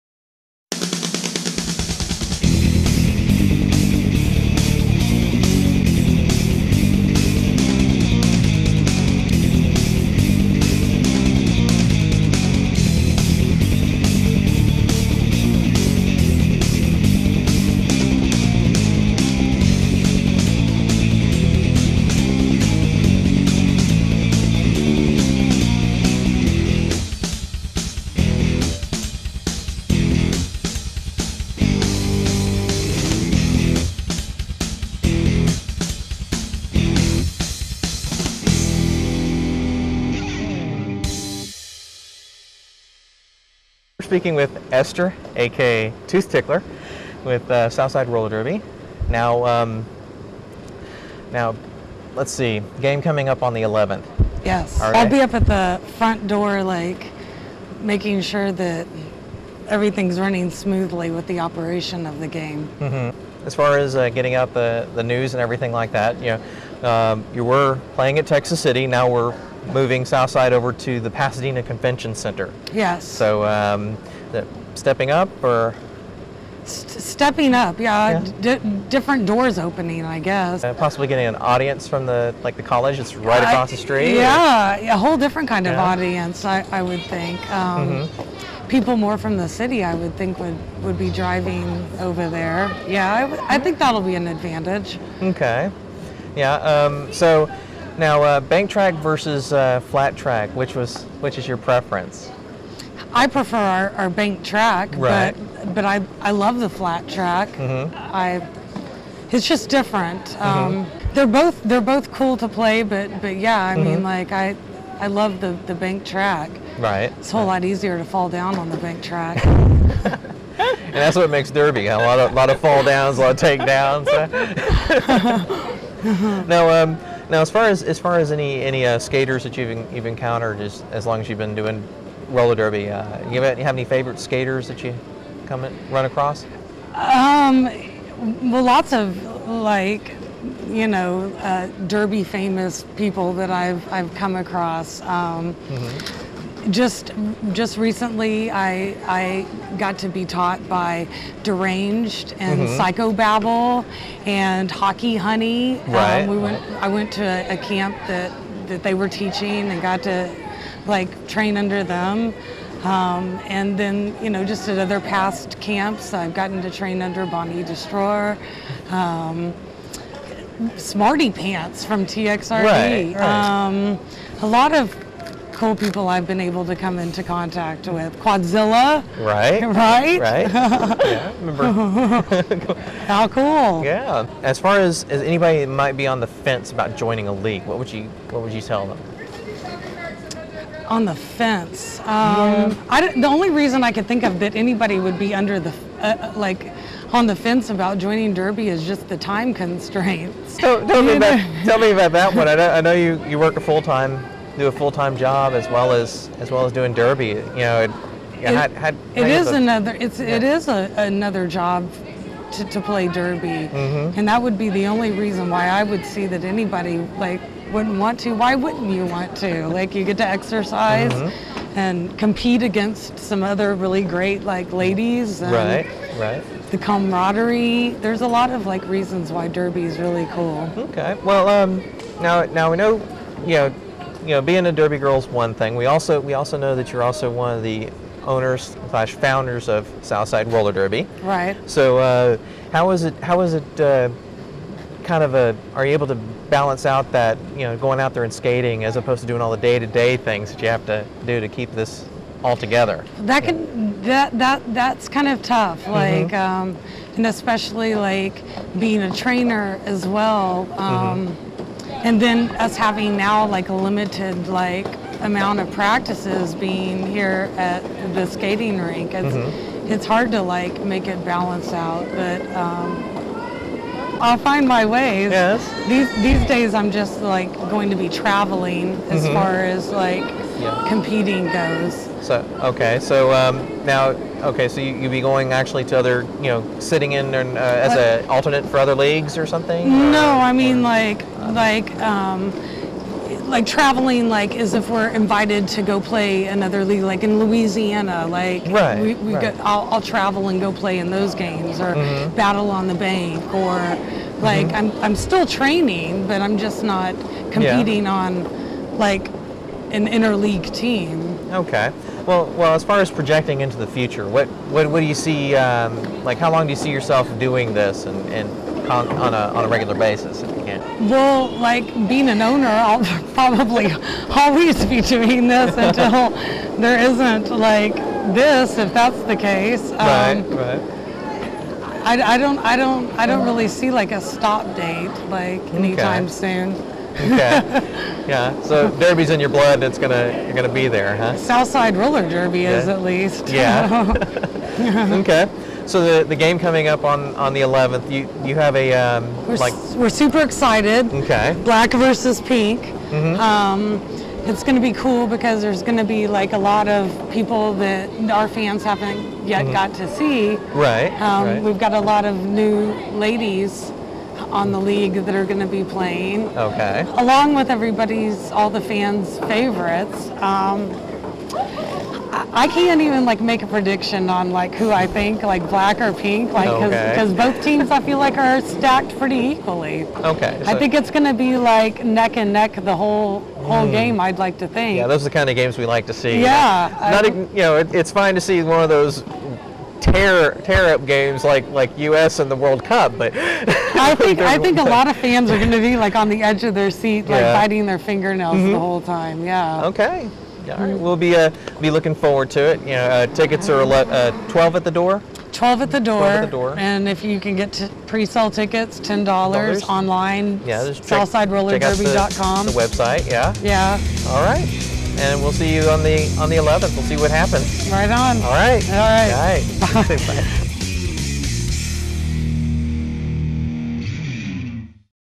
HSN_Off The Track - Interview With Esther aka Tooth Tickler
Tony Hall sits down to talk about some important topics involving the sport of Roller Derby with local rollergirls.

Featured rollergirl - Esther aka Tooth Tickler / South Side Roller Derby